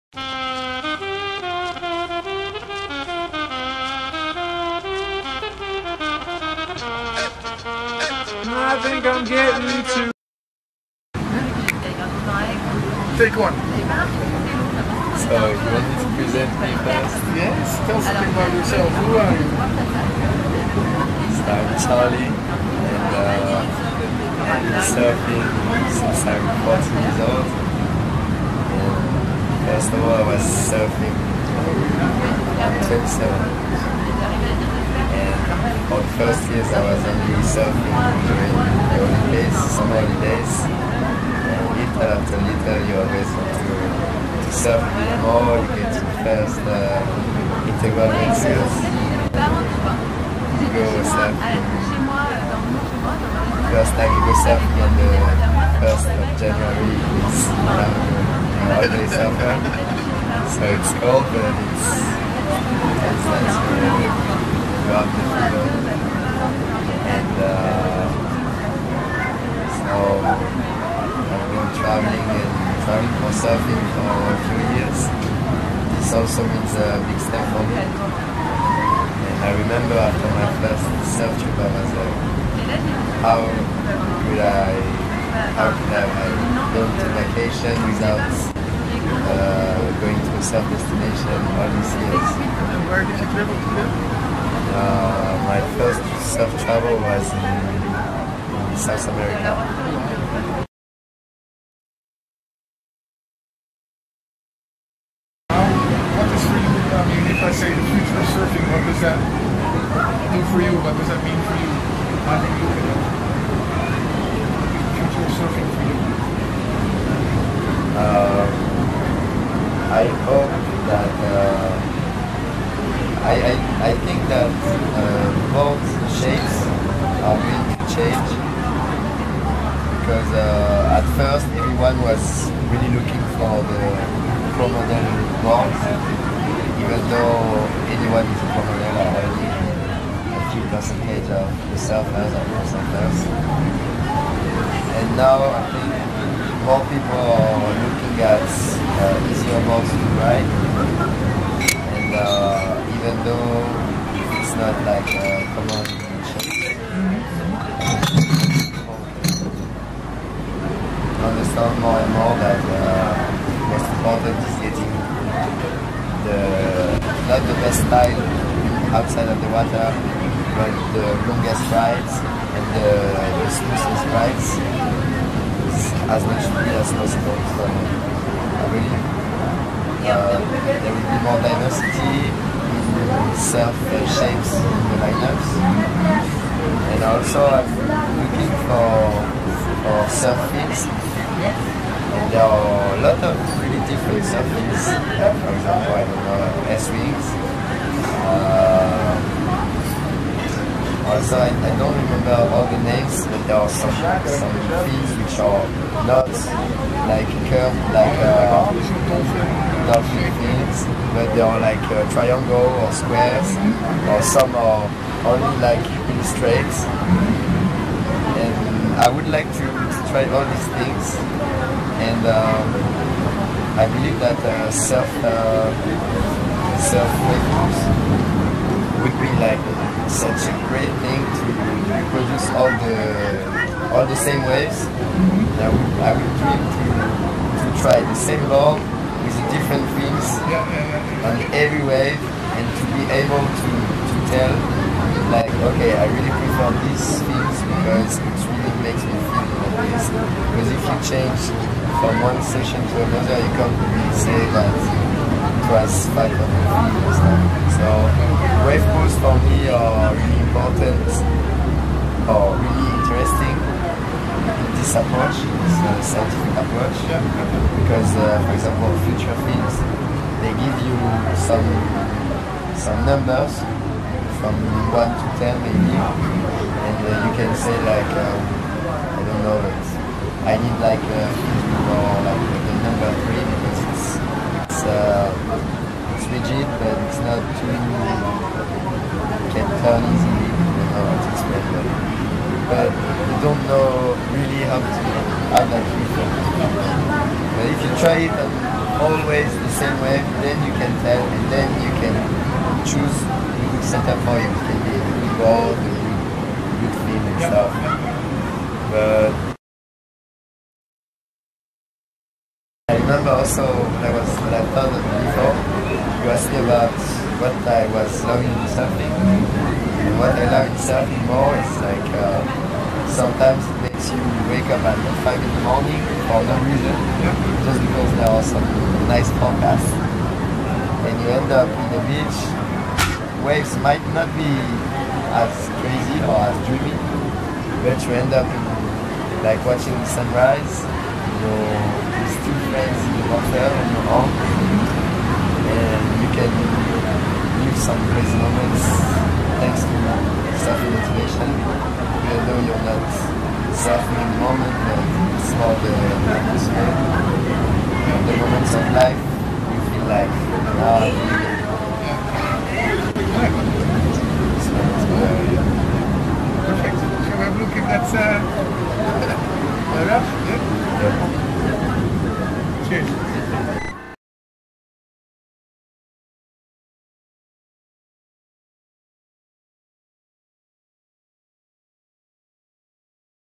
L I N E S - Charlie
Les Sables d'Olonne / Interview with Charlie Babled

How Charlie started surfing,  his ideas about the future and the joy of surfing